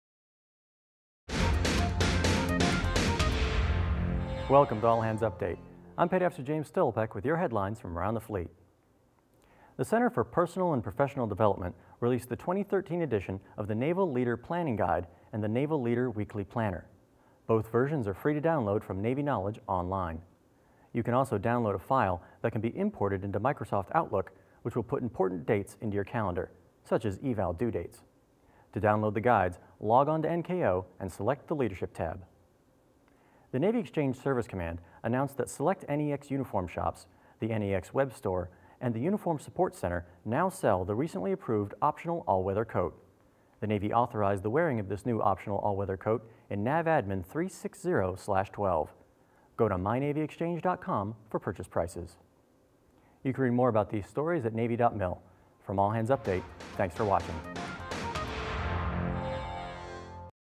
2013 Leader Planning Guides Released; All-Weather Coat Available at NEX
Headlines for January 7th, 2012: 2013 Leader Planning Guides Released; All-Weather Coat Available at NEX